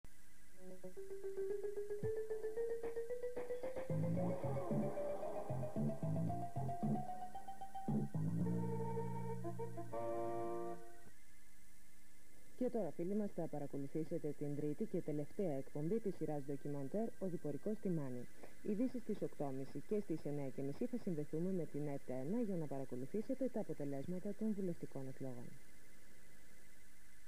ΜΑΡΙΑΝΝΑ ΜΑΥΡΕΑ ΕΡΤ2
Αναγγελία προγράμματος από την τηλεπαρουσιάστρια της ΕΡΤ-2 Μαριάννα Μαυρέα.